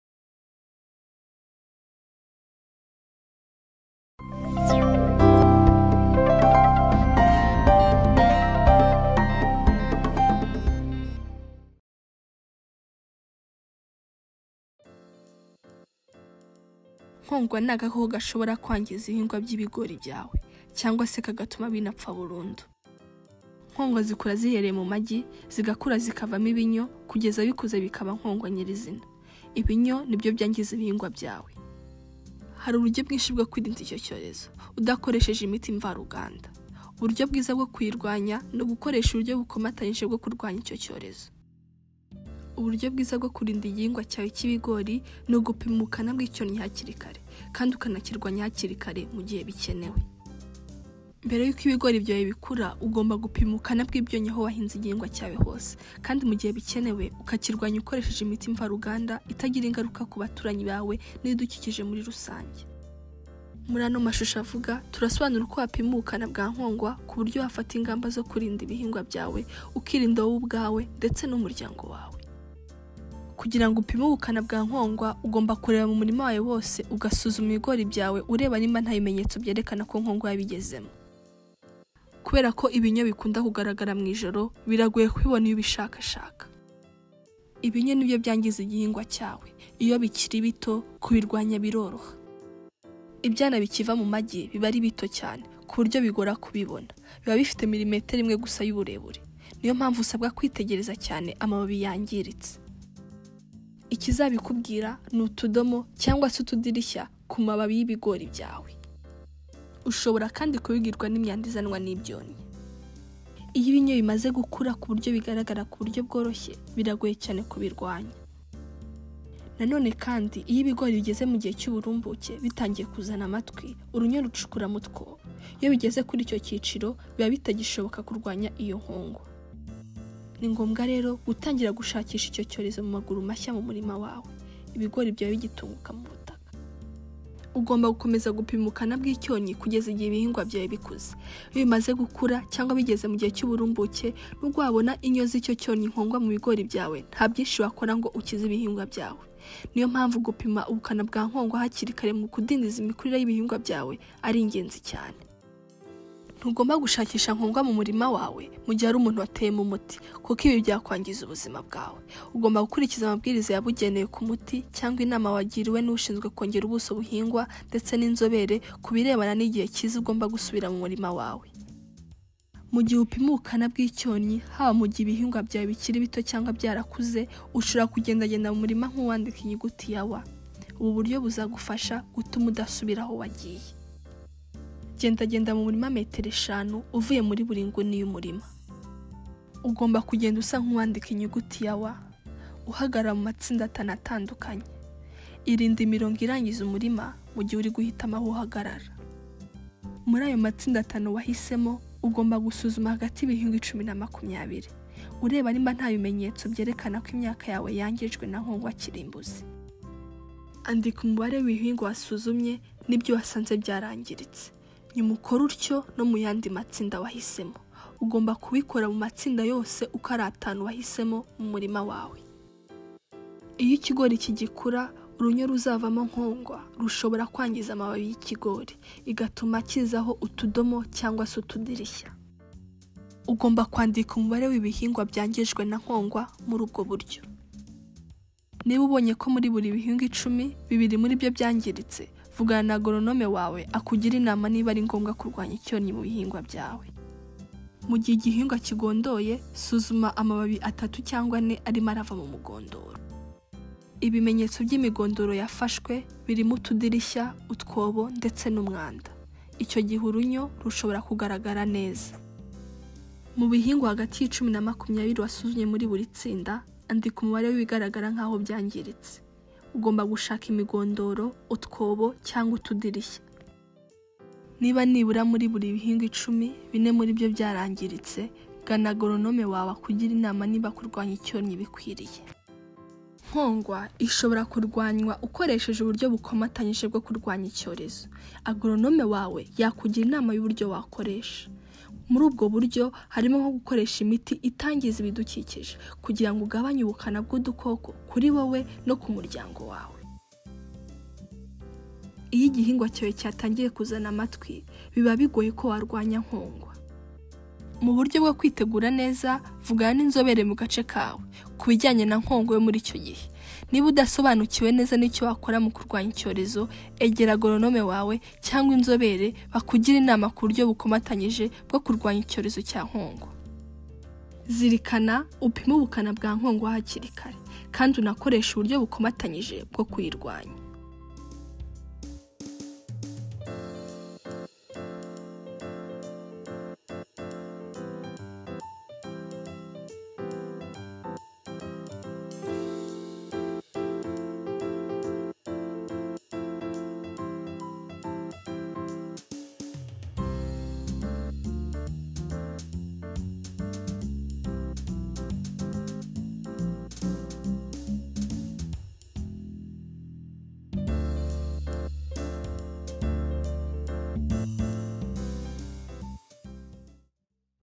Uko wamenya ukanapima ubukana bw’icyonnyi Nkongwa Kinyarwanda-Rwanda
Nkongwa , ni agakoko kangiza ibihingwa by’ibigori byawe cyangwa kagatuma binapfa burundu. Uburyo bwiza bwo kurinda igihingwa cyawe cy’ ibigori ni ugupima ubukana bw’icyonnyi hakiri kare ukanakirwanyiriza igihe. Muri ano mashusho avuga, turasobanura uko wamenya nkongwa hakiri kare, kugirango ufate ingamba zo kurinda  ibihingwa byawe , ukirinda nawe ubwawe ndetse ukanarinda n’umuryango wawe.

How to Identify and Scout for Fall Armyworm in Kinyarwanda (accent from Rwanda)

The Fall Armyworm is an insect that can damage your maize plants or even cause you to lose your whole maize crop. The best way to protect your maize crop is to scout early and treat early. In this animation, we will explain how to scout for the fall armyworm, so you can take action to protect your crop and yourself and your family.